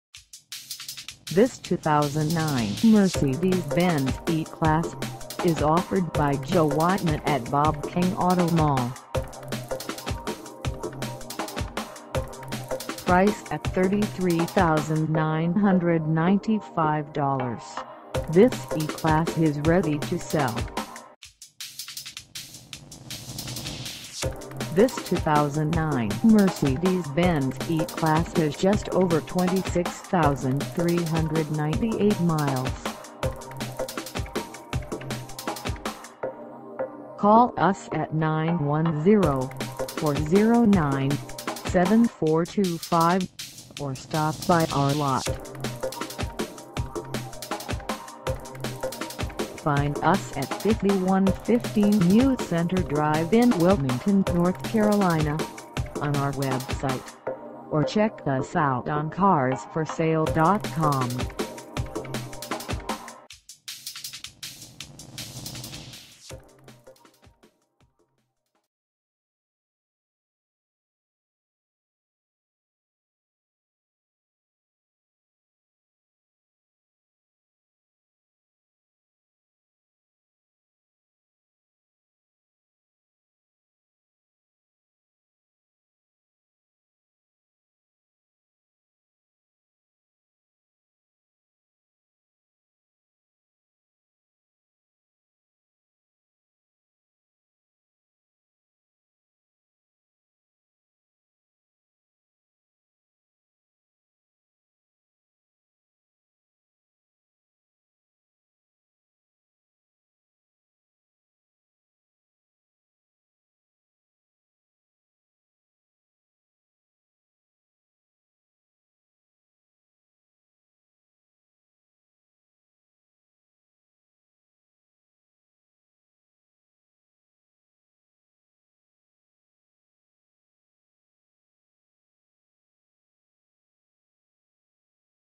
2009 Mercedes-Benz E-Class E350 - for sale in Wilmington, NC 28403
Joe Wightman at Bob King Auto Mall
5115 NEW CENTRE DR

Come test dirve this 2009 Mercedes-Benz E-Class E350 for sale in Wilmington, NC.